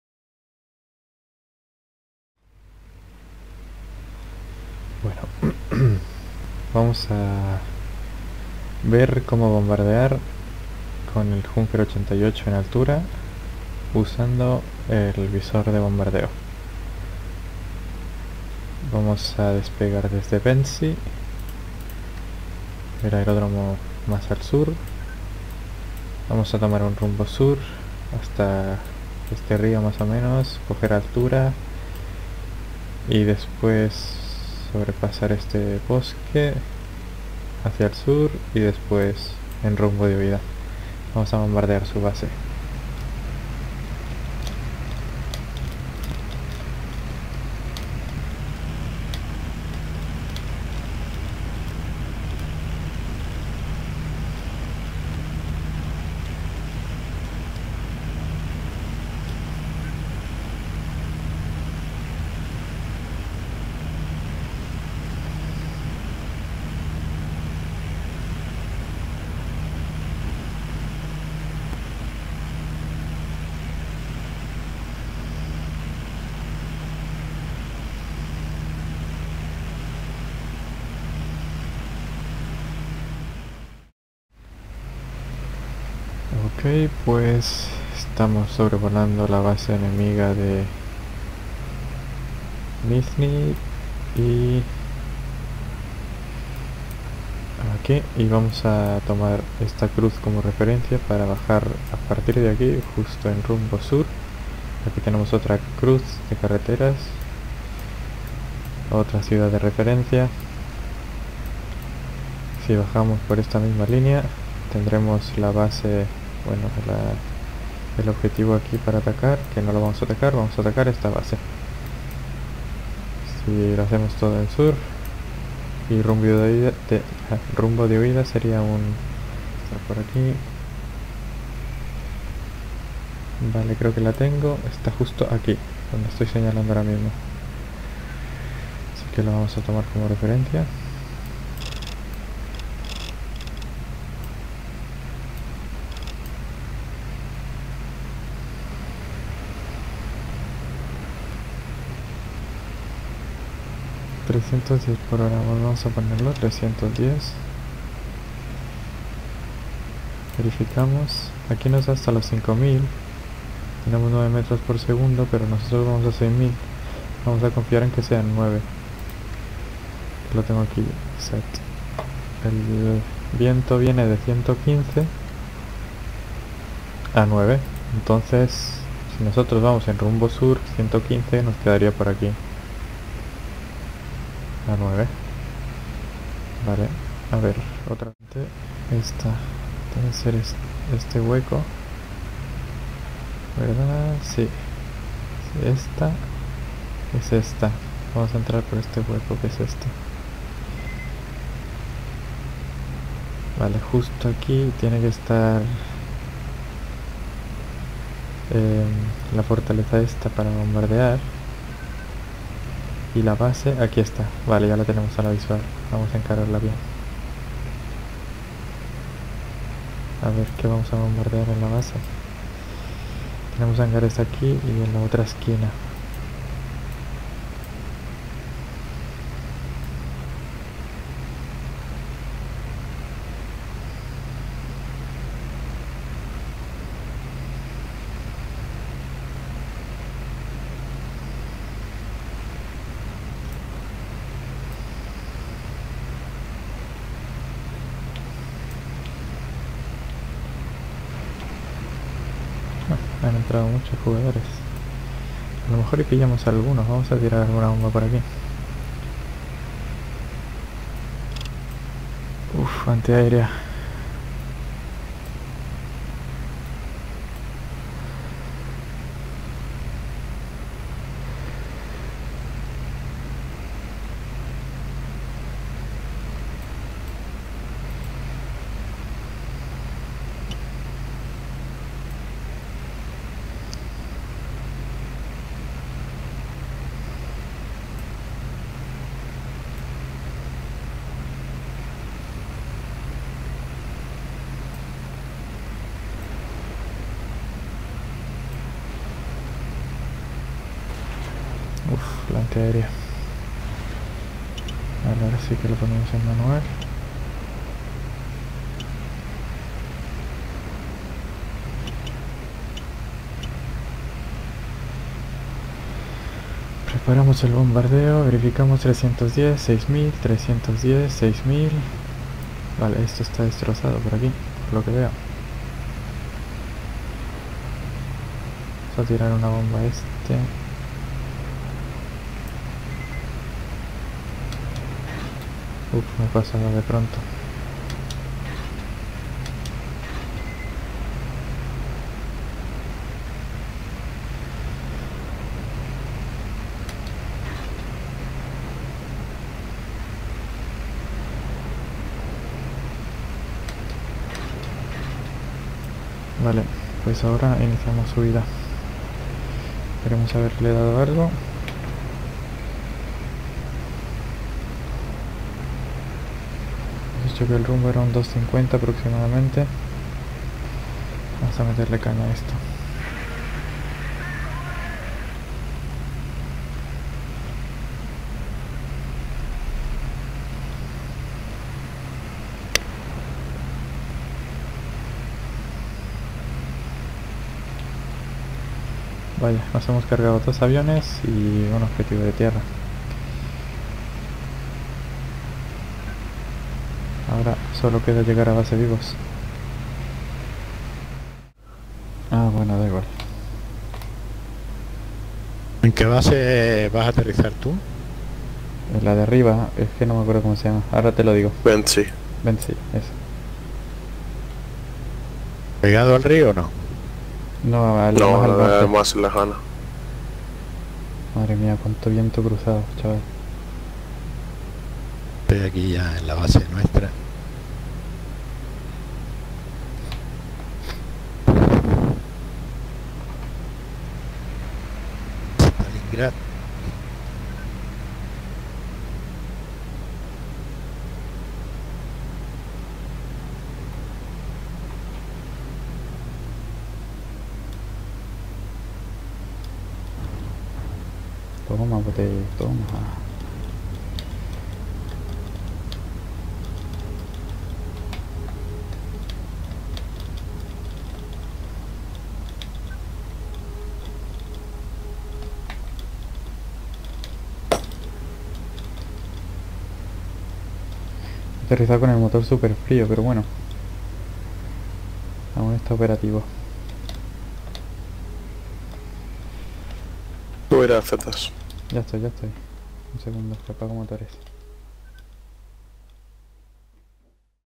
Bombardeo en altura
Bombardeo usando el visor de bombardeo con el Ju88